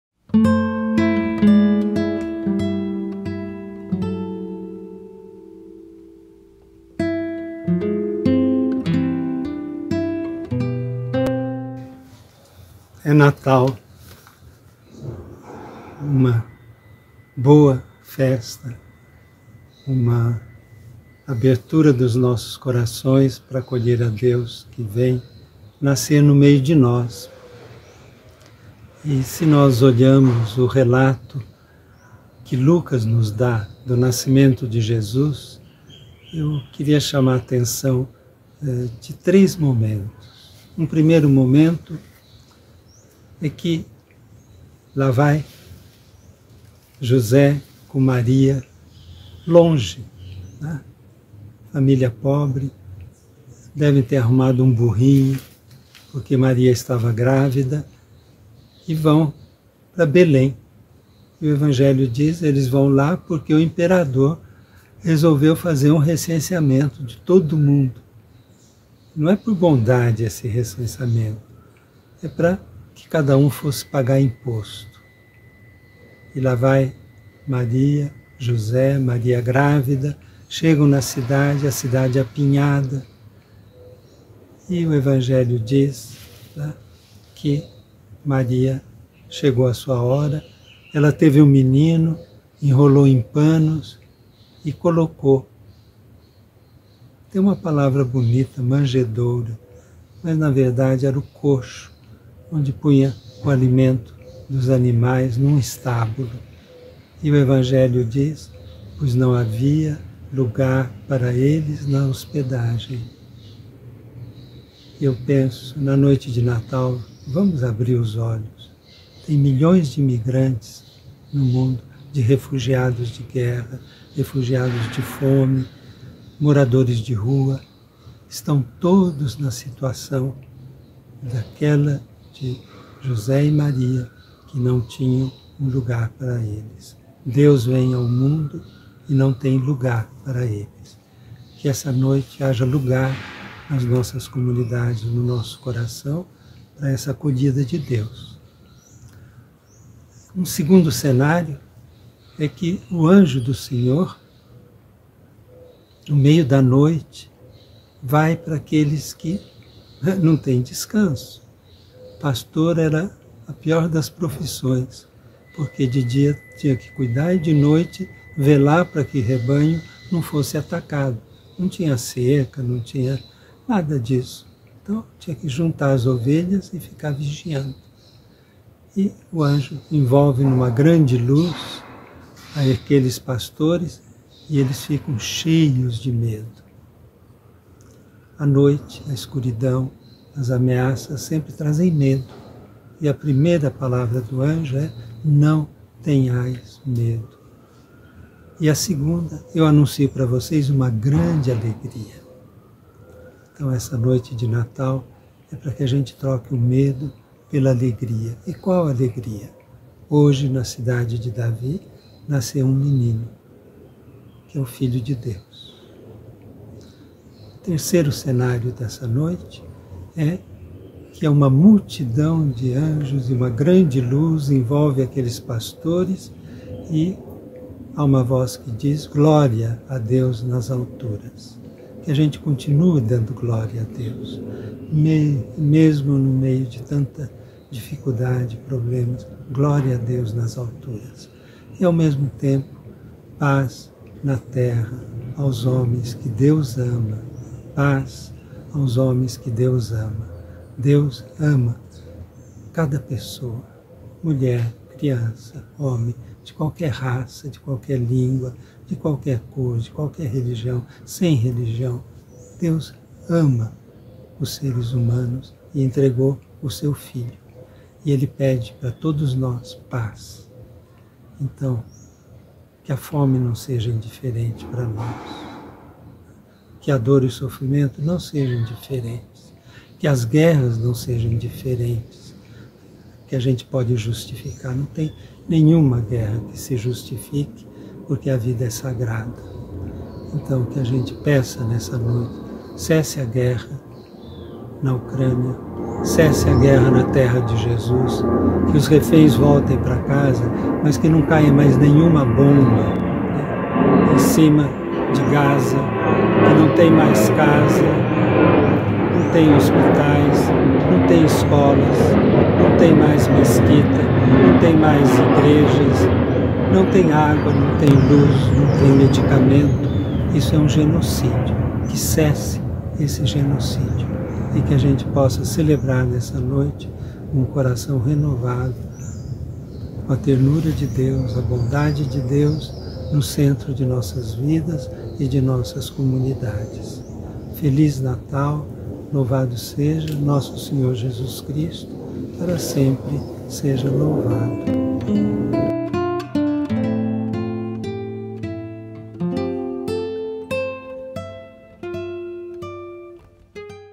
BEOZZO: É Natal, “Gloria a Deus no mais alto dos céus e paz na terra aos homens por Ele amados”
HOMILIA: É Natal, “Gloria a Deus no mais alto dos céus e paz na terra aos homens por Ele amados” (Lc 2, 1-14)

Abramos nosso coração para acolher Jesus, que se fez humano no seio de Maria e nasceu pobrezinho em Belém, como anunciou o anjo do Senhor aos pastores: “Não tenhais medo! Eu vos anuncio uma grande alegria, que o será para todo o povo: hoje na cidade de Davi, nasceu para vós um Salvador, que é o Cristo Senhor. Isto vos servirá de sinal: Encontrareis um recém nascido envolvido em faixas e deitado numa manjedoura” (Lc 2, 10-11). Que troquemos o medo pela grande alegria.

Dobremos nossos joelhos em ação de graças diante da manjedoura e, em silêncio, adoremos este dom e este mistério, junto com Maria, José e os pastores, meditando nas palavras do evangelho de João: “Deus tanto amou o mundo, que entregou seu filho único, para quem crer, não pereça, mas tenha a vida eterna. Deus não enviou o seu Filho ao mundo, para julgar o mundo, mas para que o mundo se salve por meio dele” (Jo 3, 16-17). “Não havia lugar para eles na estalagem” (Lc 2, 7) e, por isso, Deus veio ao mundo num estábulo. Que sejamos solidários com os milhões de migrantes, refugiados, exilados, moradores em situação de rua, que são também rejeitados em nossos países e em nossas cidades, pois para eles não há um lugar em nossas sociedades construídas encima da desigualdade e da exclusão. O anjo do Senhor dá glória a Deus e anuncia paz na terra aos homens por Ele amados. Todas as mulheres, homens e crianças de qualquer raça, cor, credo religioso ou sem religião, são amados por Deus. Que não deixemos entrar no nosso coração discriminação em relação a qualquer ser humano, porque é estrangeiro, está na pobreza, doente ou abandonado. Que a fome não nos seja indiferente, que a dor e o sofrimento humano não nos sejam indiferentes, que as guerras não nos sejam indiferentes. Neste Natal, peçamos fervorosamente para que cesse a guerra entre Rússia e Ucrânia, que haja cessar¬-fogo imediato na terra de Jesus, com o retorno dos reféns a seus lares e que pare o massacre da população de Gaza, bombardeada dia e noite, há dois meses e privada de suas casas, escolas, postos de saúde, hospitais, mesquitas e igrejas, de água, comida, luz, medicamentos e assistência médico-sanitária. Que nossas relações sejam presididas pela ternura e pelo cuidado, pela compaixão, perdão e solidariedade, para que possamos celebrar o Natal, envolvidos pela graça e bondade de Deus reveladas no nascimento de seu Filho Jesus.